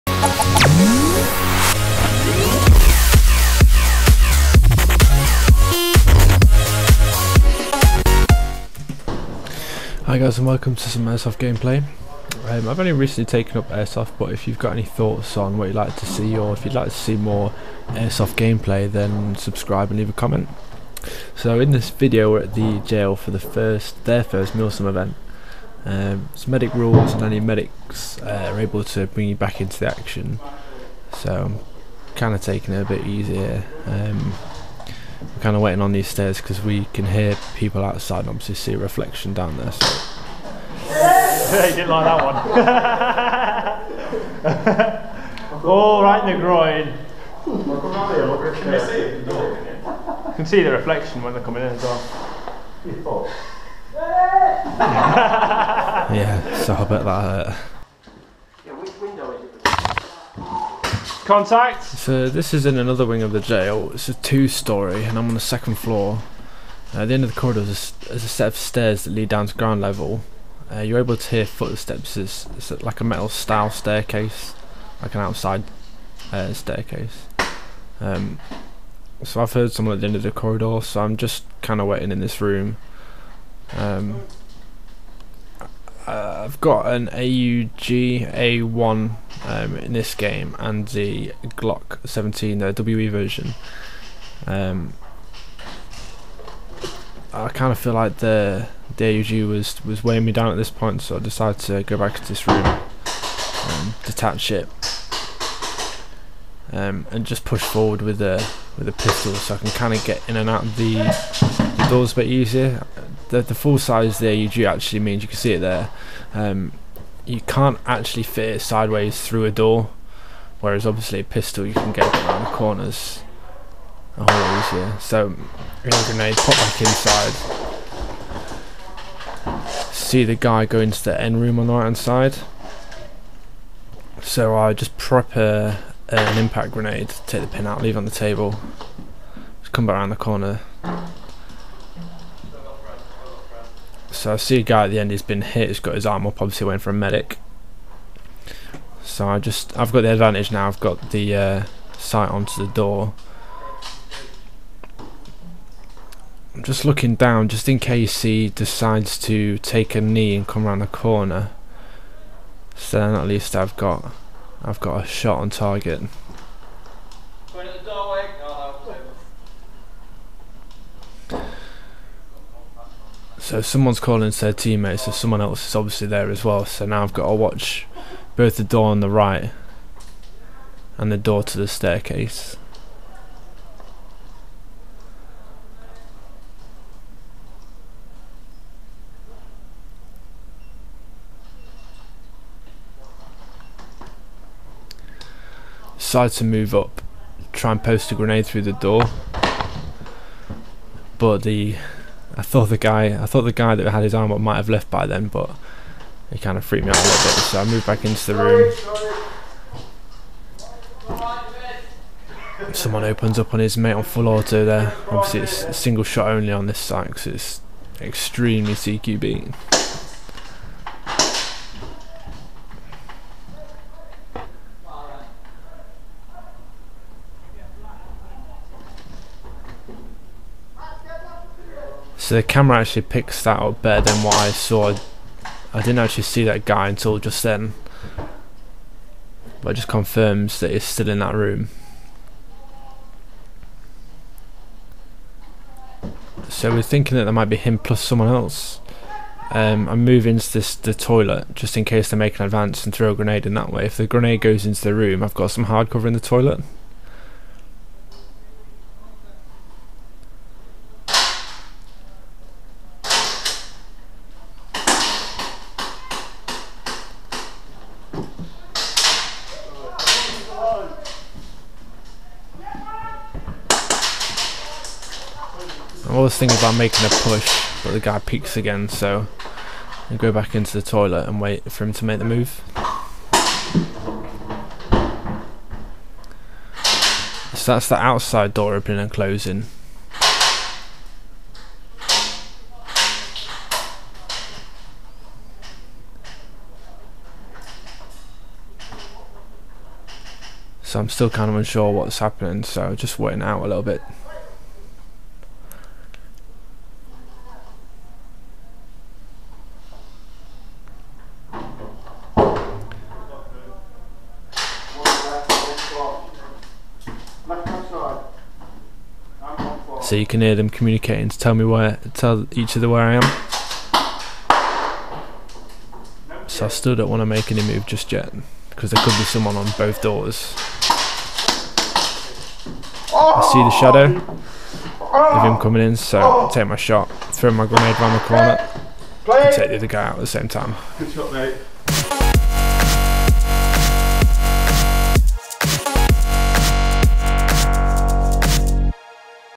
Airsoft Prison Milsim | The Gaol | WE G17
Airsoft WE G17 gameplay at an urban site. The Gaol

GWEGGLES

PRINCESS

Airsoft Addict